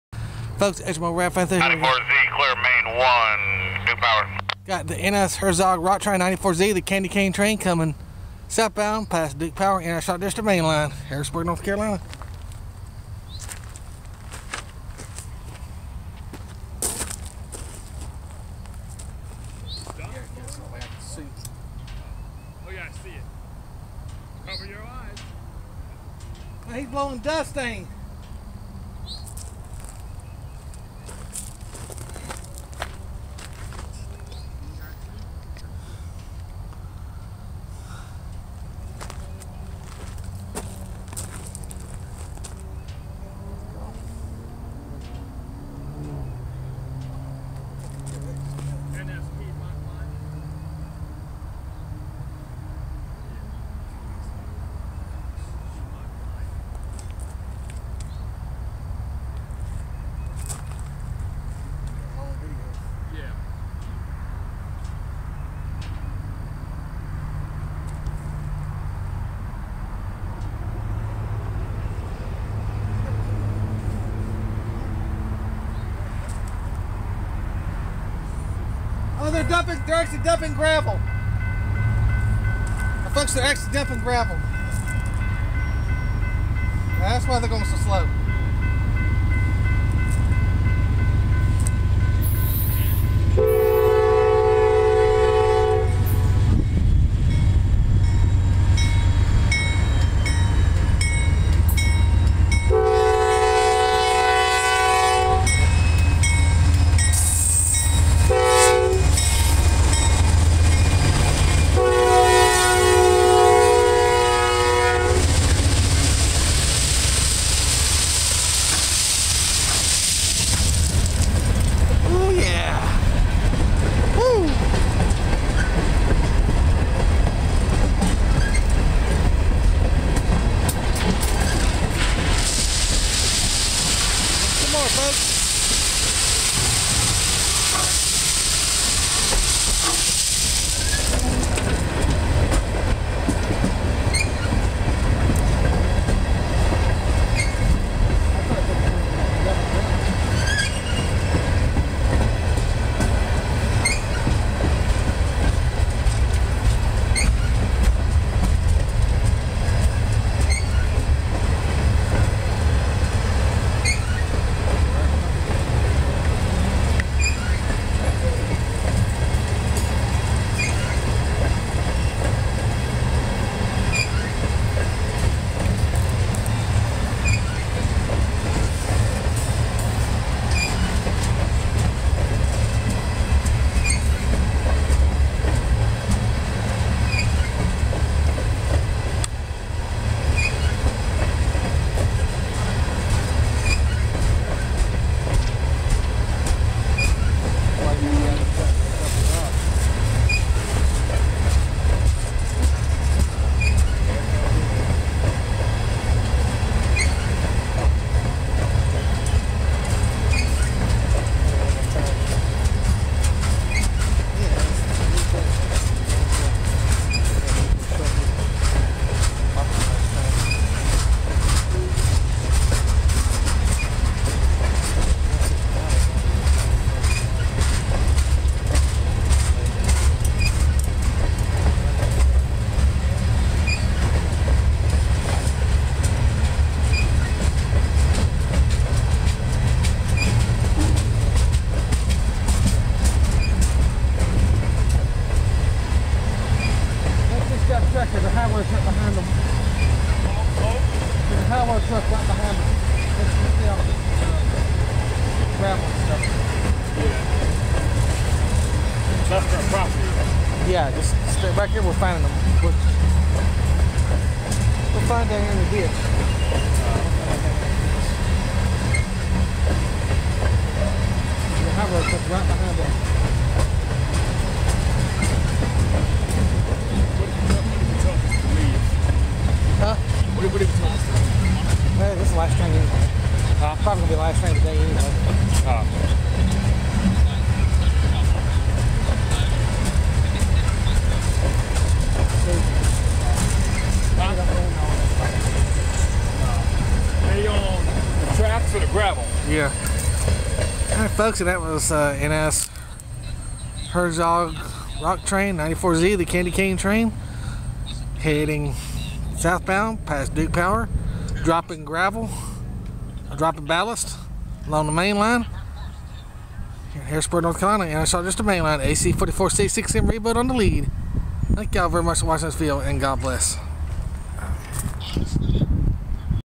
NS Ballist Train 94Z With NS Leader 4265 At Harrisburg NC On The NS Charlotte Mainline. 4-3-2021
NS Rock Ballist Train 94Z Heads Southbound Past Duke Power In Harrisburg North Carolina Along University City Blvd Dumping Rock Ballist On Track 1 On The NS Charlotte District Mainline With NS Rebuild AC44C6M Locomotive 4265 Leading The Way.